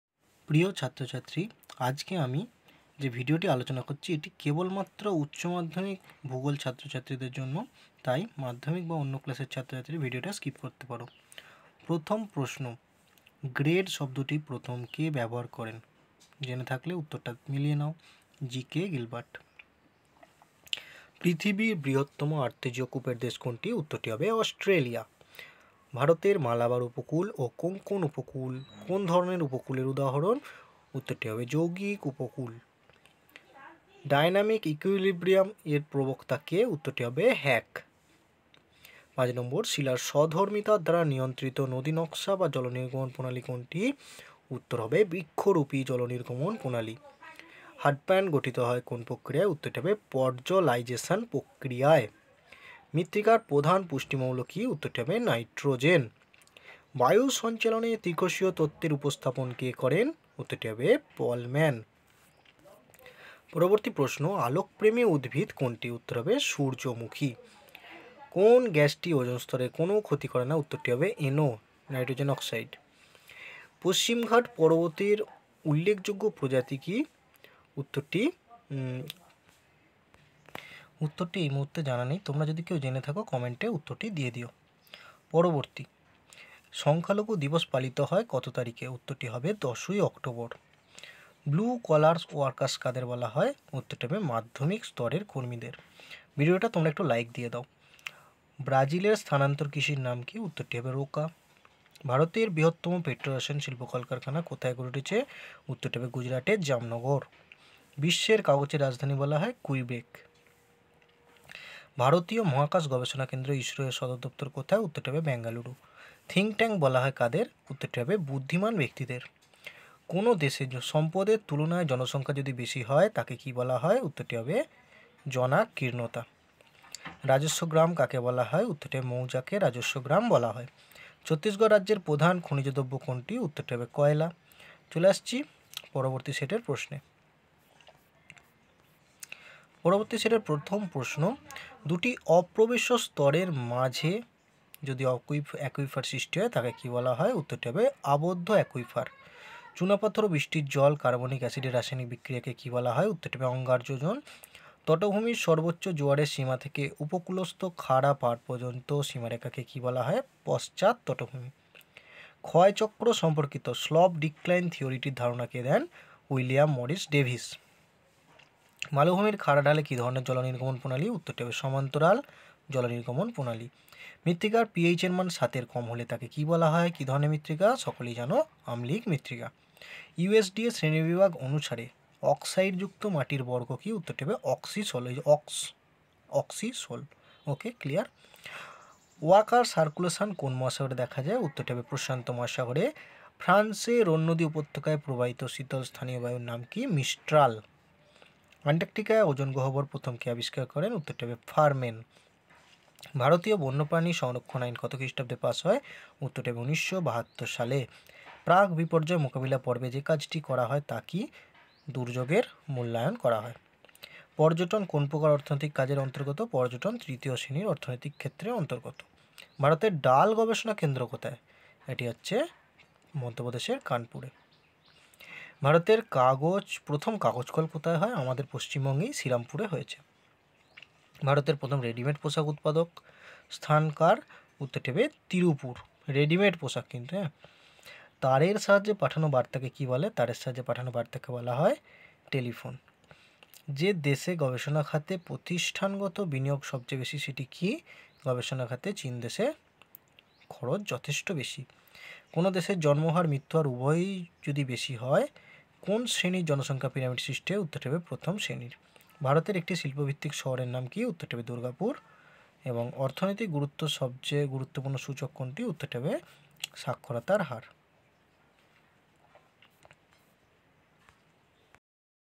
HS/ মডেল সেট আলোচনা-১ / short questions/ class 12 /
উচ্চ মাধ্যমিক , ক্লাস টুয়েলভ এর ছাত্র-ছাত্রীদের জন্য ভূগোলের বিভিন্ন মডেল প্রশ্ন উত্তর আলোচনা করা হয়েছে যেগুলি প্র্যাকটিস করলে তোমরা 2023  ফাইনাল পরীক্ষায় অনেক প্রশ্ন এর থেকে কমন পাবে।

⭐তোমার সাফল্যই আমার আনন্দ.

⭐আমার TELEGRAM CHANNEL NAME:- @Soupal007